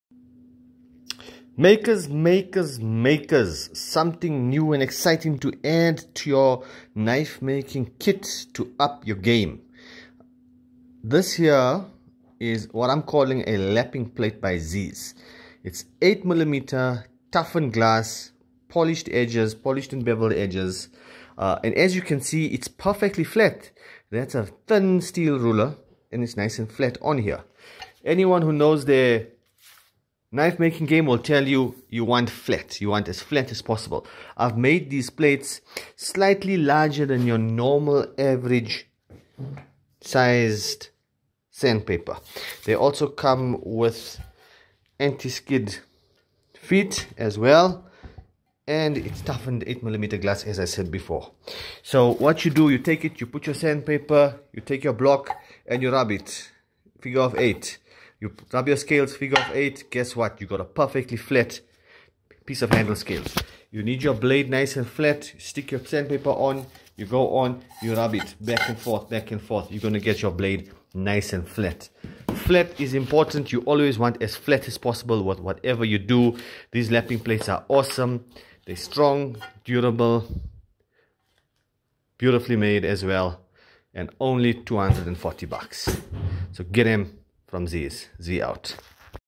Lapping plates now in stock
Lapping plates now in stock - 8mm toughened glass with polished and beveled edges..Get your stuff FLAT. ONLY R240.00